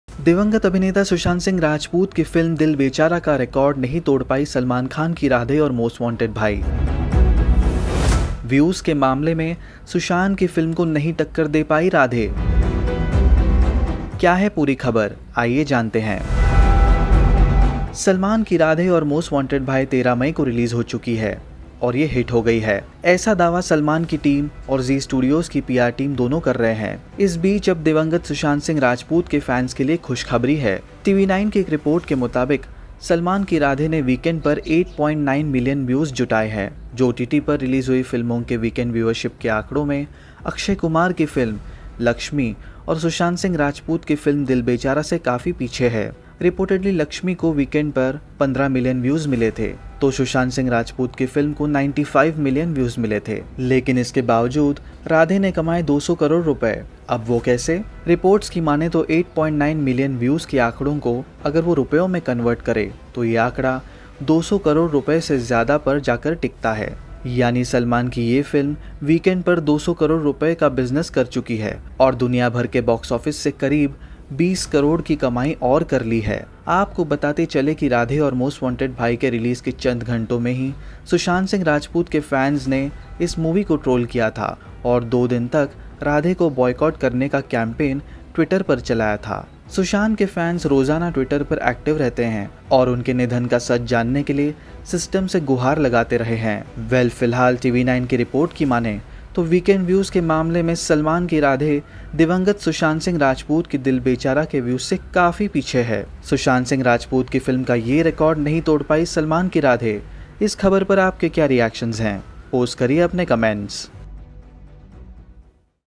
Salman Khan's Radhe Loses Against Sushant Singh Rajput's Film | Watch How
Salman Khan VS Sushant Singh Rajput: A News report claims that Radhe Your Most Wanted Bhai has faced massive failure in front of Late actor Sushant's film Dil Bechara. Watch the story.